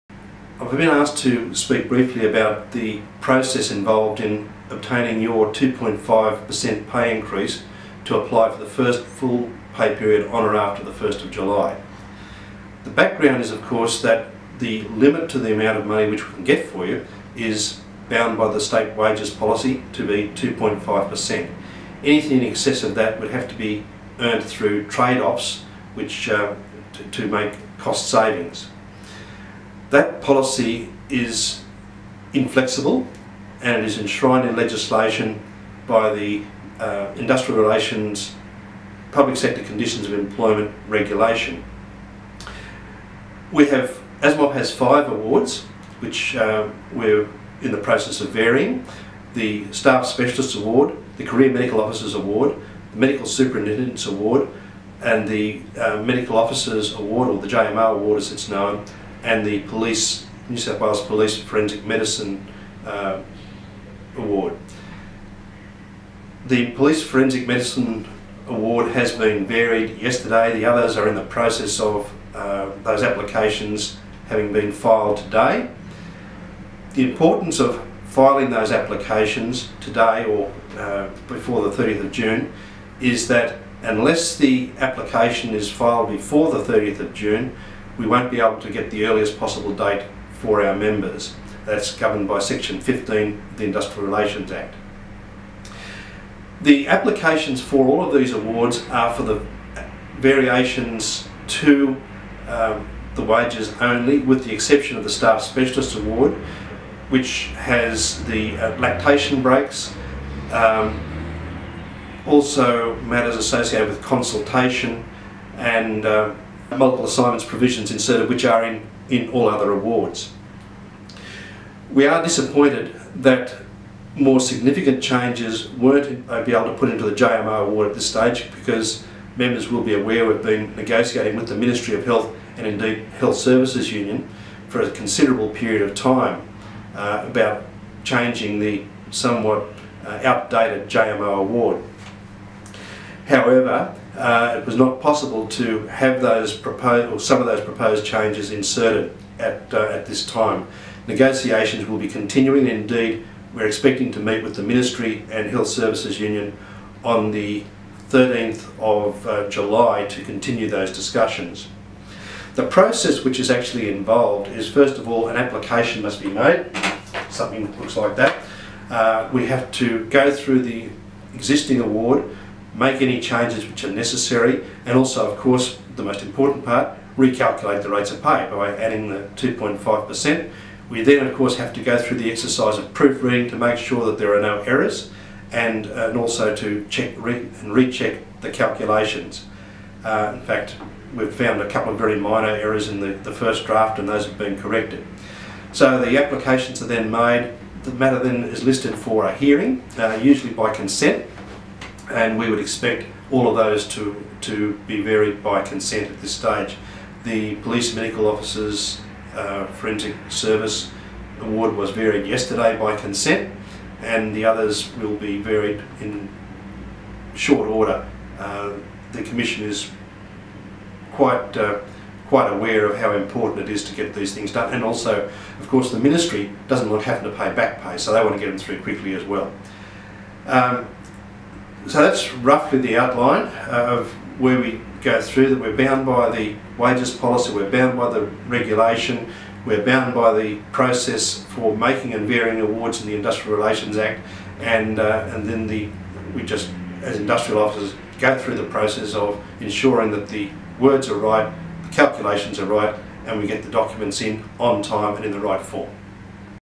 160628 BM 2 5 Wage Increase v1
Bob Morgan, Senior Industrial Officer from ASMOF explains how we seek the 2.5% Wage increase every year.